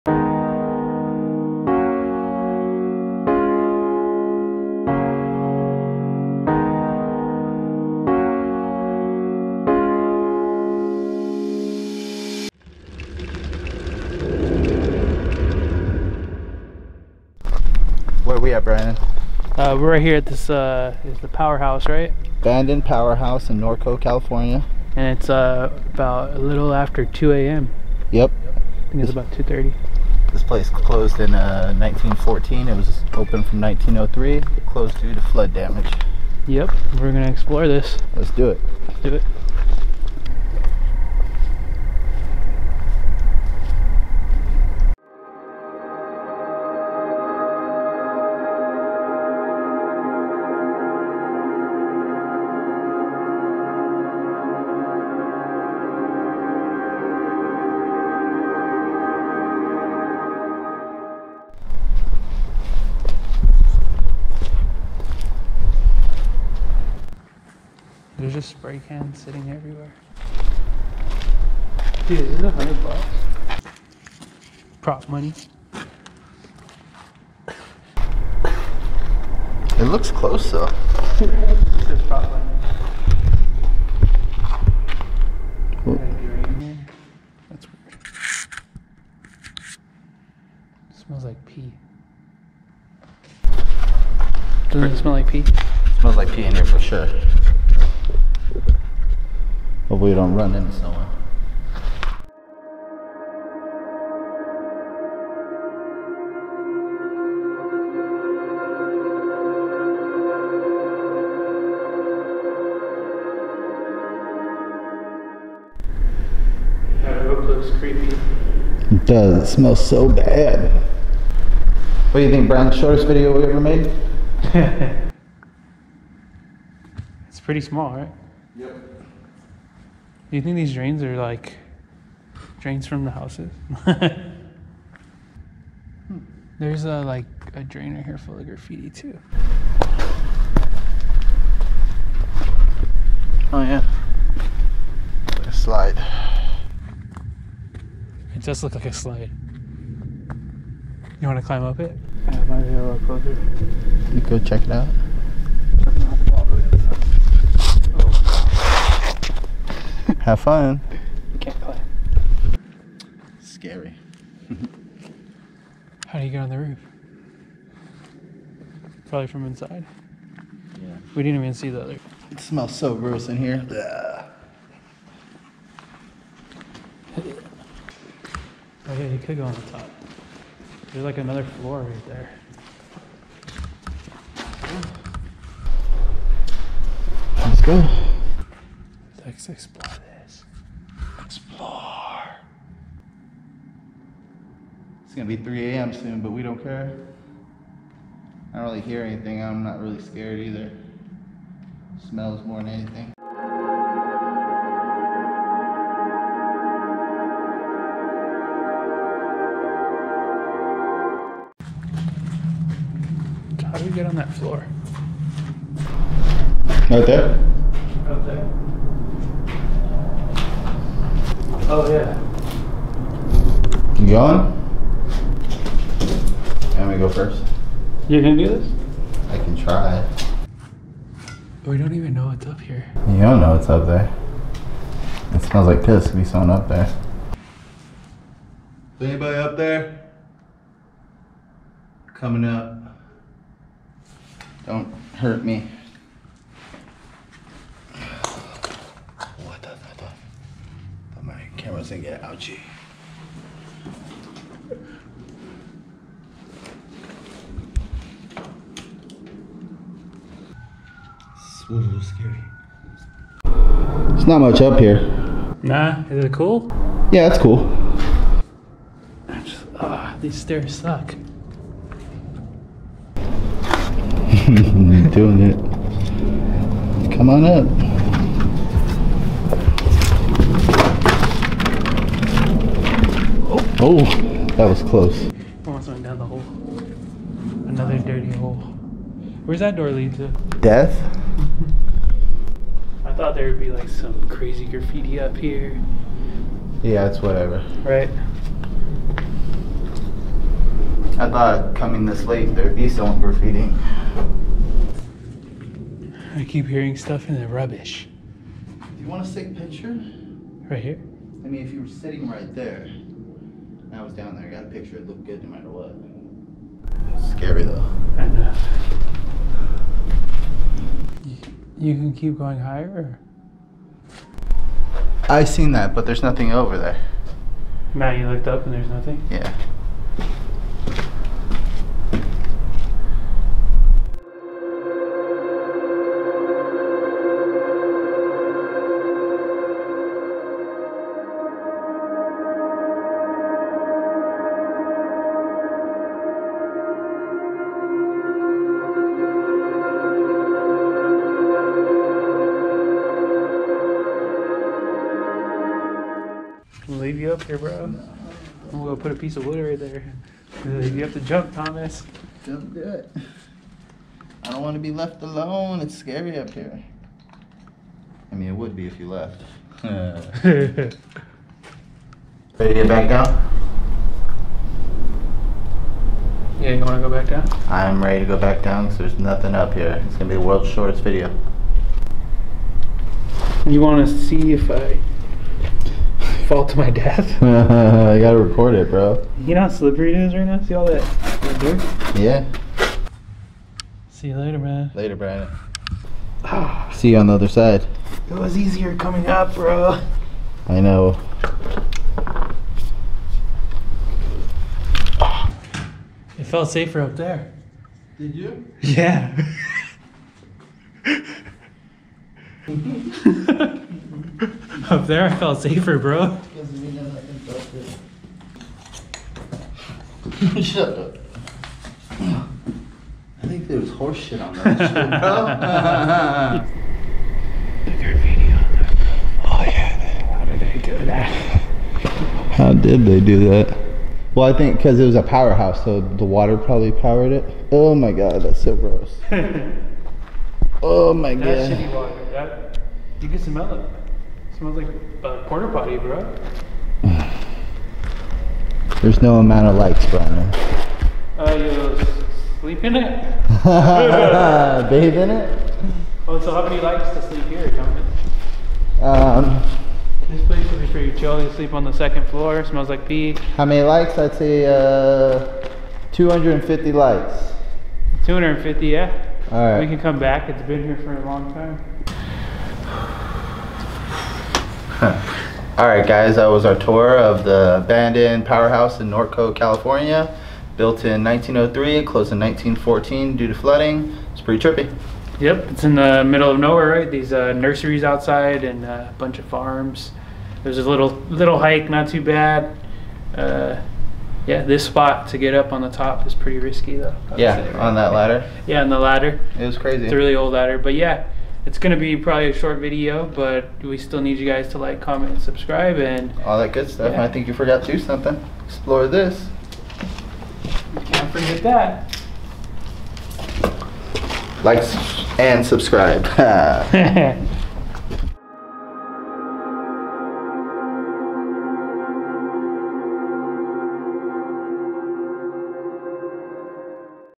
Norco's Abandoned Powerhouse At Night [Money Found]
Follow Thomas and Brandon from ExploredThis as they explore the old abandoned Powerhouse in Norco, CA.  Watch as they find money (prop) and signs of people just being there.

Norco Powerhouse, also known as the Pedley Powerhouse, was built in 1903 and is located between farmlands and neighborhoods. The Powerhouse closed in 1914 due to flood damage.
The site is now known mostly for urban exploring as well as paintball and music videos.

Did You Like Our Video? Share It On Social Media Hit The Share Button Above Or Copy This